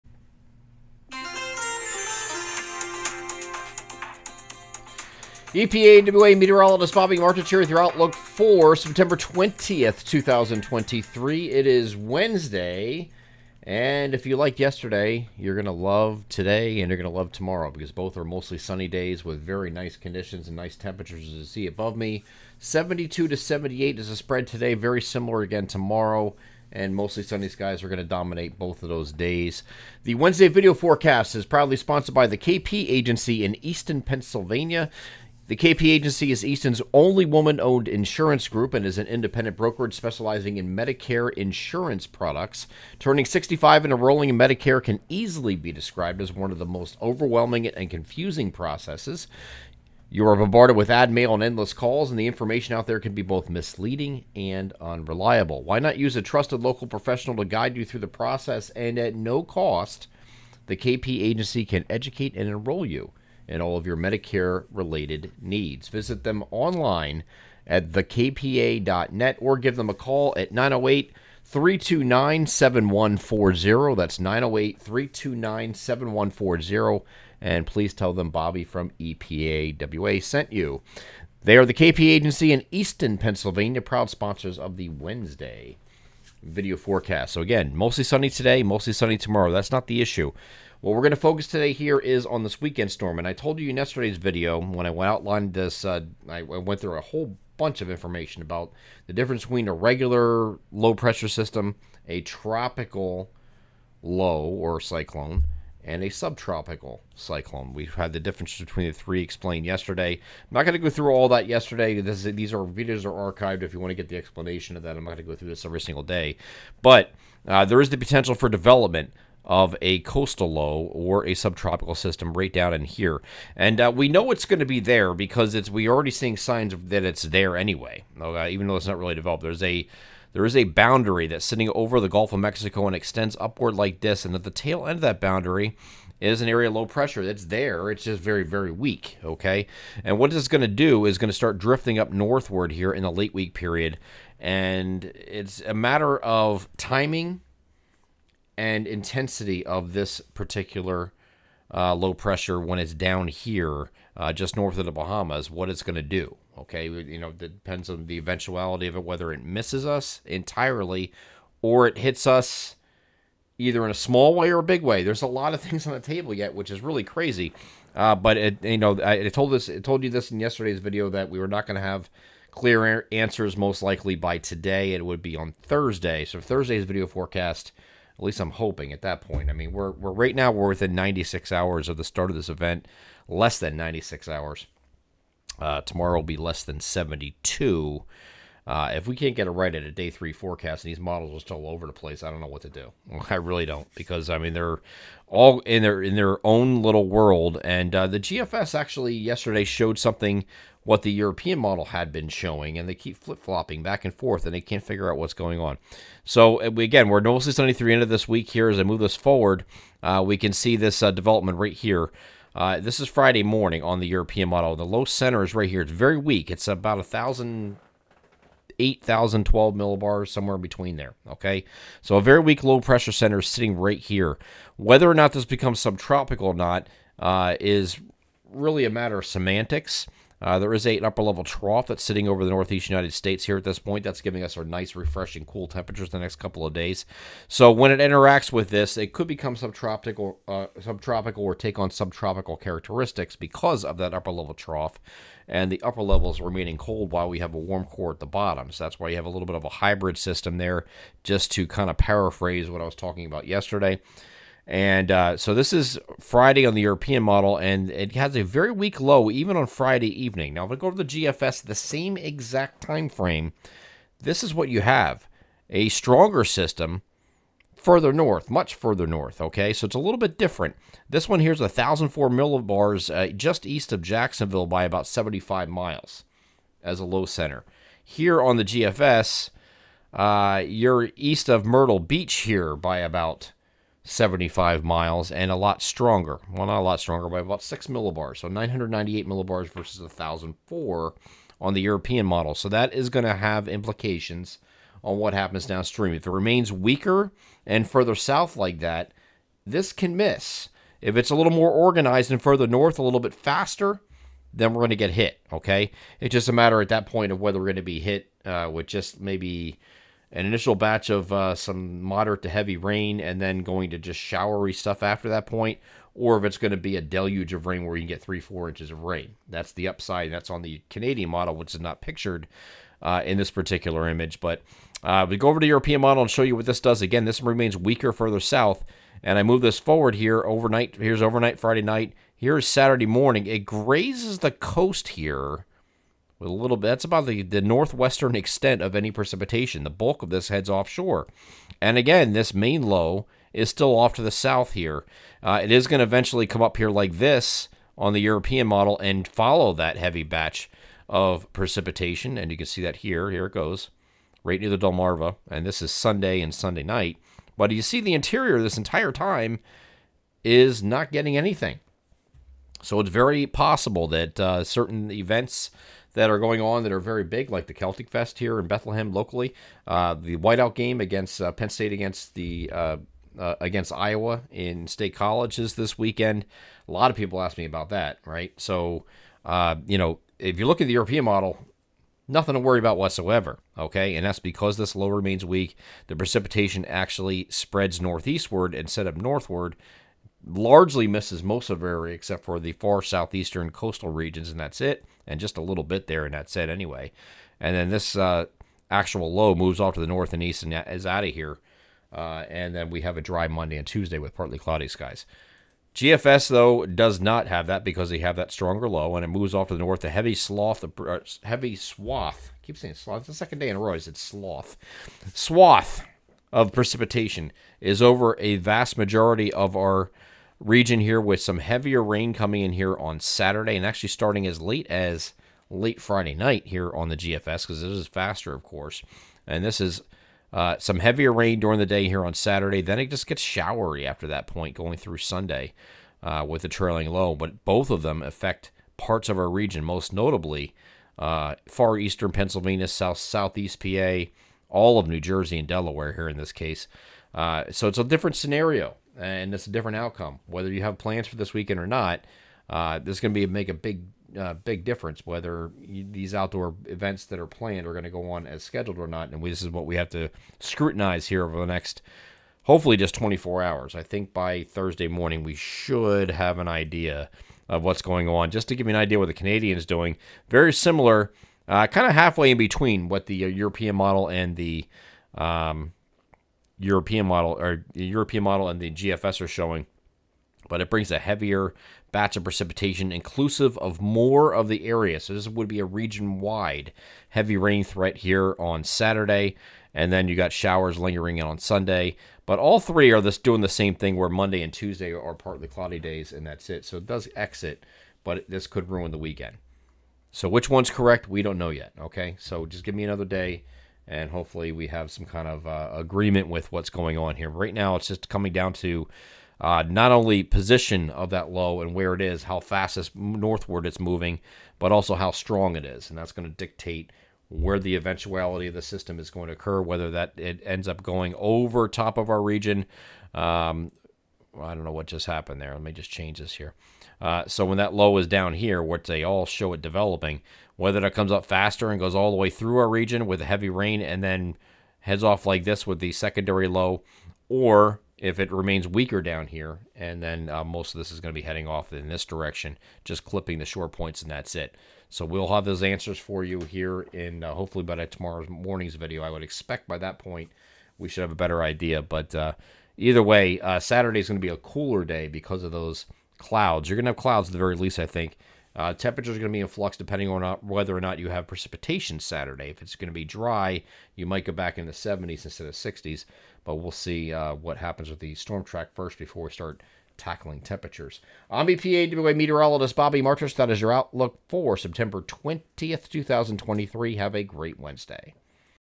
Wednesday September 20th, 2023 video forecast
EPAWA's 9/20 and week ahead outlook, covering:
■ A couple more stellar days thru end of work week
■ Coastal/subtropical storm update...yes or no?
■ Drawing the scenarios for a wet or dry weekend